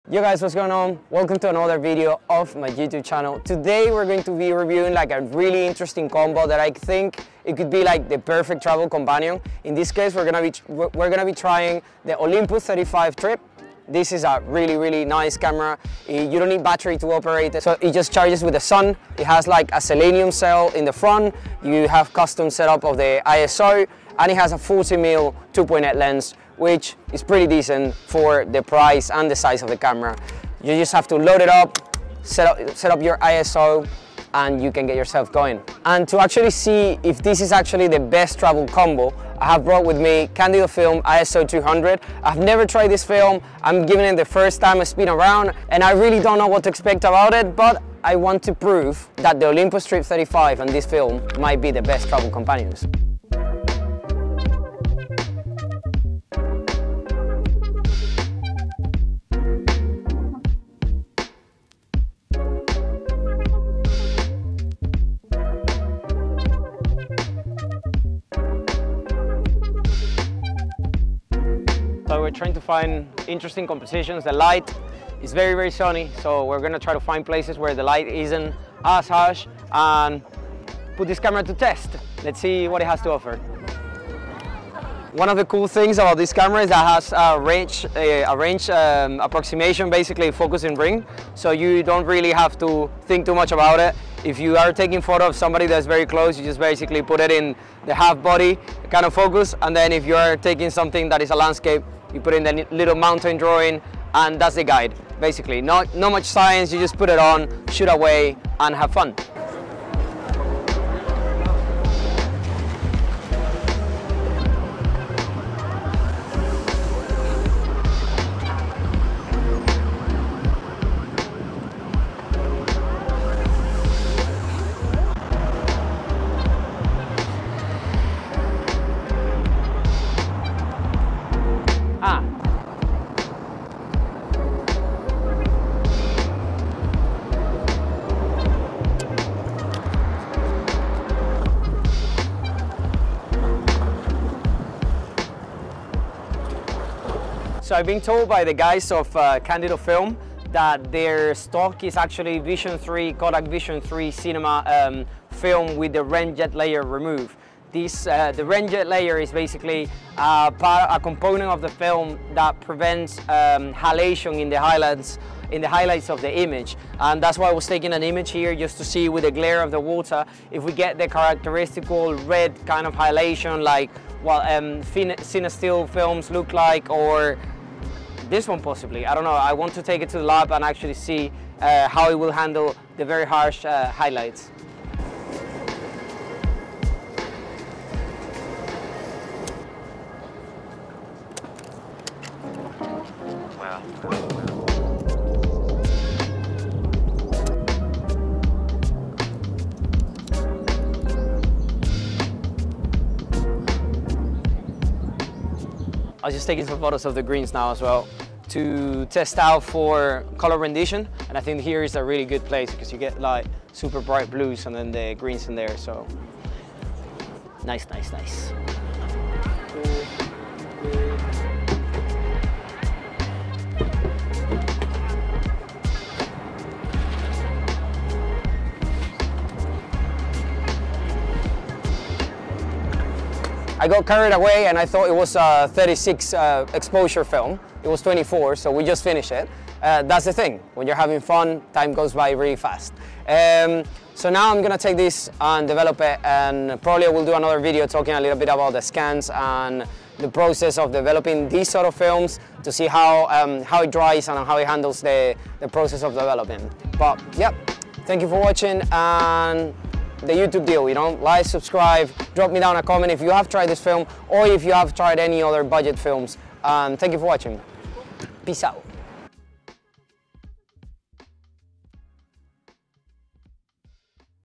The Perfect Travel Film Photography Combo: Olympus Trip 35 Camera & Candido 200 Film Review
Hey everyone! In this video, I'm sharing my thoughts on the Olympus Trip 35 camera and Candido 200 film - the perfect travel film photography combo! I recently took this camera and film on a sunny day adventure around central London and was blown away by the results.

Throughout the video, I talk about the features and benefits of the Olympus Trip 35 camera, including its compact size and easy-to-use controls. I also discuss the beautiful color reproduction that the Candido 200 film produces. I share some of my favorite shots from my day out and give you some tips on how to use this combo to take amazing travel photos.

If you're looking for a casual and easy-to-use film camera for your travels, the Olympus Trip 35 might be the perfect choice for you. And if you're looking for a film that produces beautiful, vivid colors, Candido 200 is definitely worth trying out.

So grab a cup of coffee, sit back, and join me on this casual and fun adventure as we explore the beauty of film photography together.